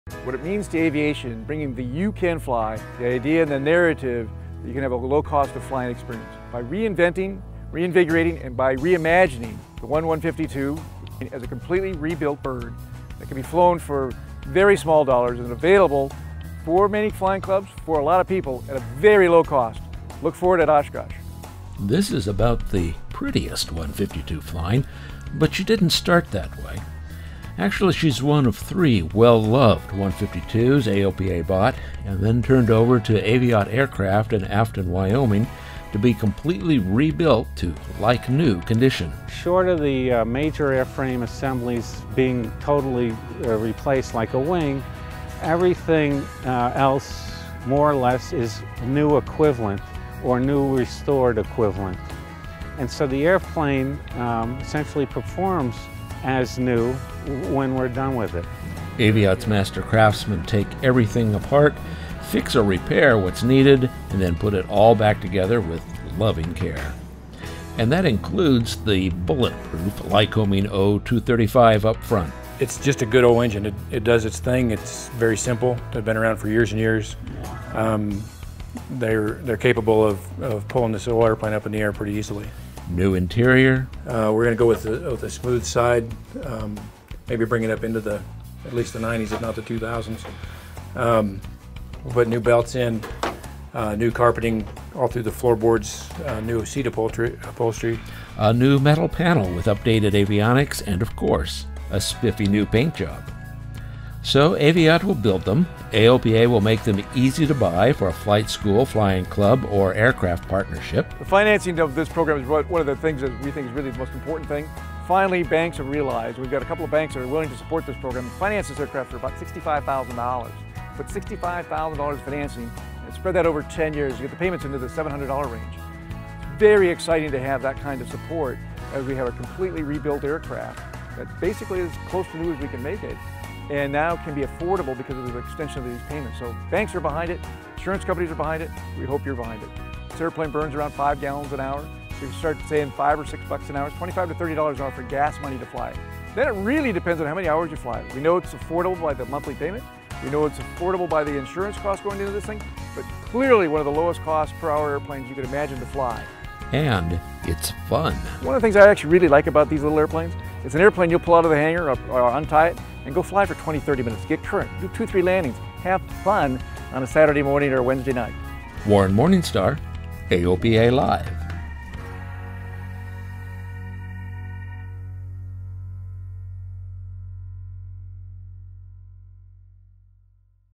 C152 Reimagined
AOPA tests new ways to open door to aviation with "Reimagined Aircraft" such as a Cessna 152 that can be co-owned and operated for as little as $65/hour, including fuel.